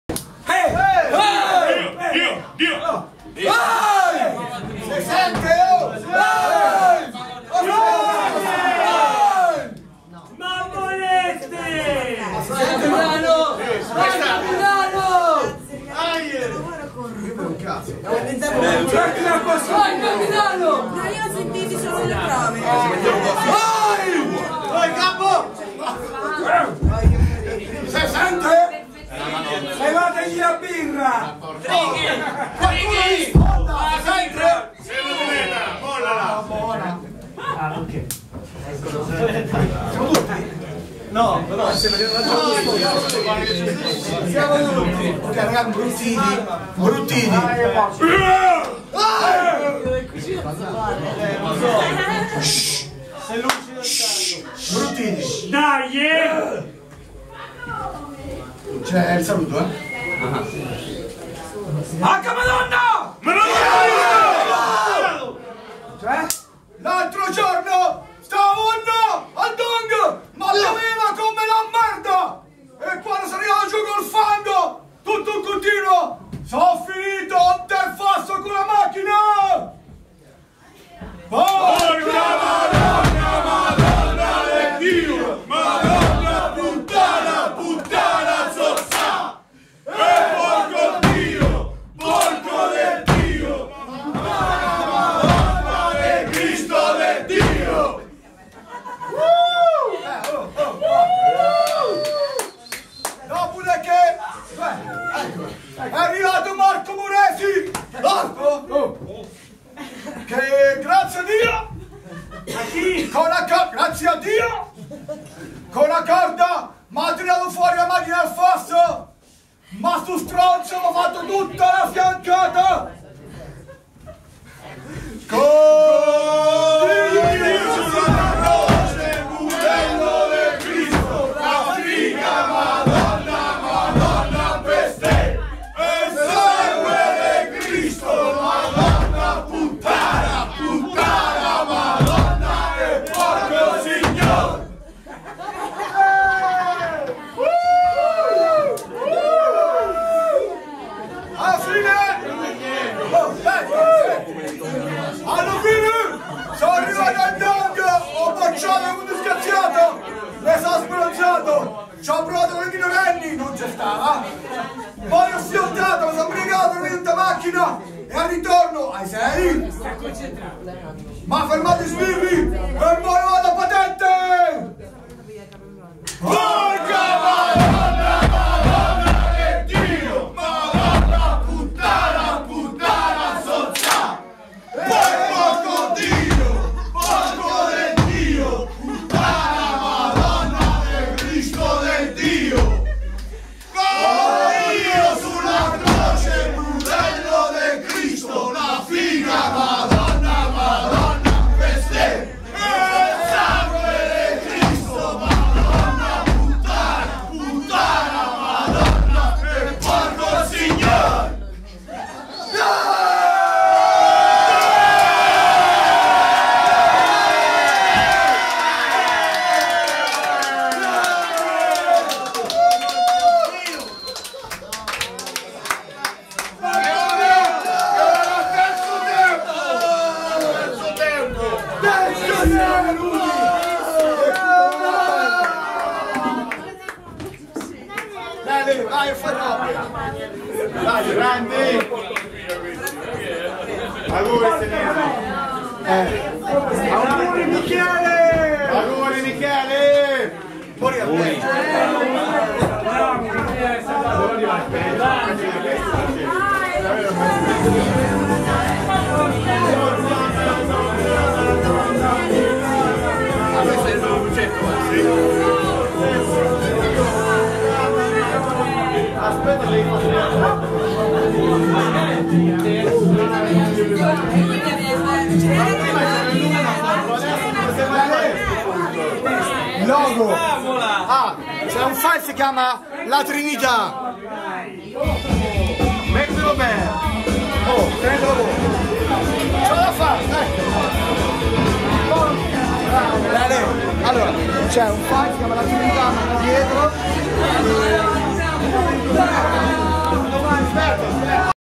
HAKA MADONNAH UNCUT
(pron.: acamadonnancàt)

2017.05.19
durante la prima serata dell'UTUTUTUTUTUTU festival
(presso il Circolo Dong di Montefiore di Recanati MC)
tra i vari eventi si è tenuta anche la prima mondiale
dell'HAKA MADONNAH,
qui finalmente vedibile in versione integrale.